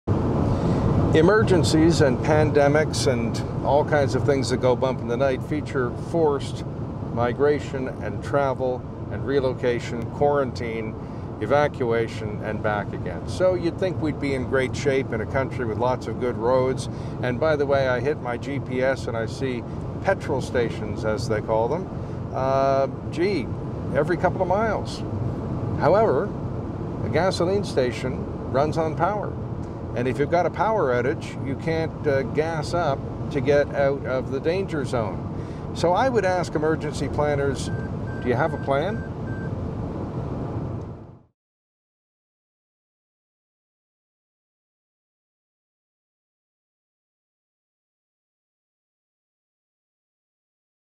Are we really prepared? | COVID-19 Pandemic, Health, Safety and Emergency Planning
Emergencies, pandemics, quarantines, and emergencies can force travel and relocation. With doing that, are we truly prepared and has your city thought through emergency plans?

ORDER THE NEW BOOK - Emergency! Quarantine, Evacuation, and Back Again